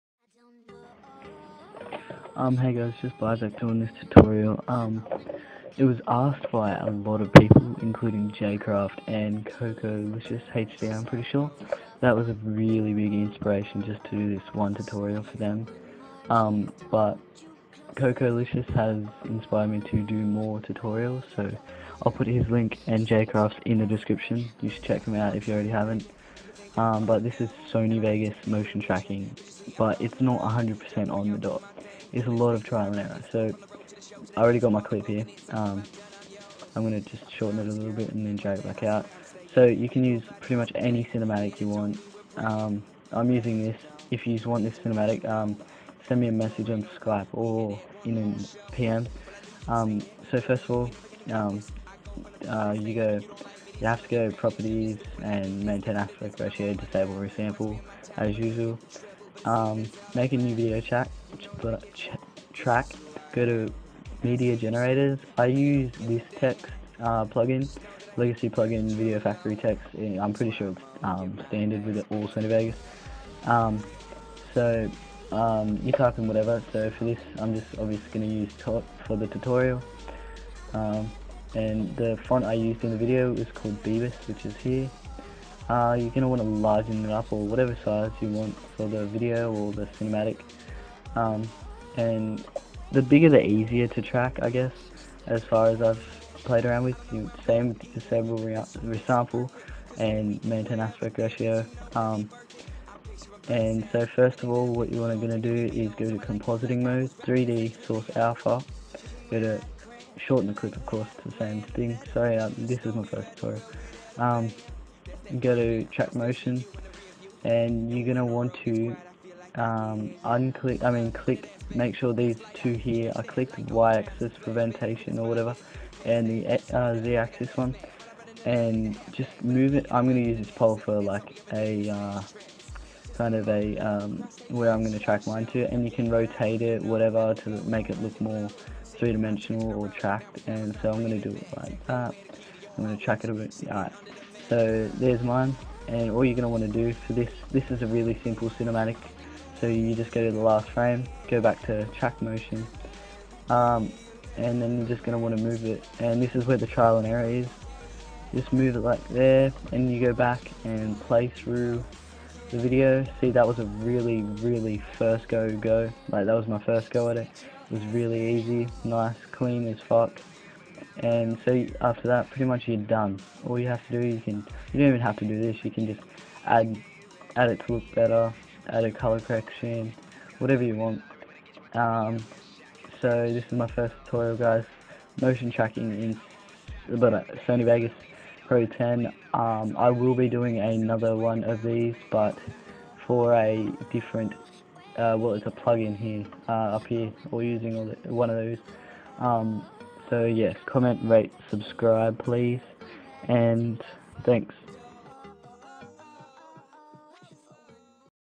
HOW TO MOTION TRACK IN SONY VEGAS! [SONY VEGAS TUTORIAL]
Hey guys, this guy literally begged me to upload this tutorial. (this is REPLICATING motion tracking, not real motion tracking!)
enjoi as always and be sure to check out his channel!